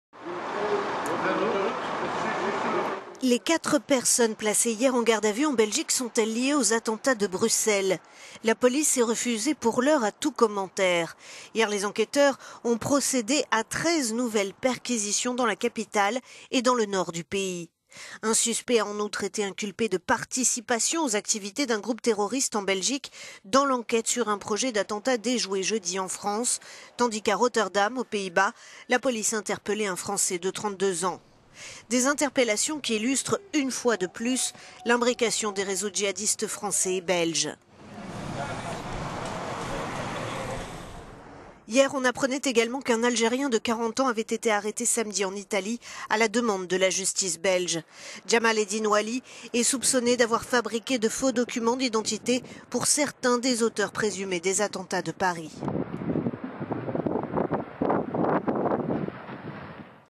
Treize nouvelles perquisitions en Belgique
Les quatre personnes placées hier en garde à vue en Belgique sont-elles liées aux attentats de Bruxelles ? La police s'est refusée, pour l'heure, à tout commentaire. Hier, les enquêteurs ont procédé à 13 nouvelles perquisitions dans la capitale et dans le nord du pays.

Un suspect a en outre été inculpé de "participation aux activités d'un groupe terroriste" en Belgique dans l'enquête sur un projet d'attentat déjoué jeudi en France, tandis qu'à Rotterdam, aux Pays-Bas, la police interpellait un …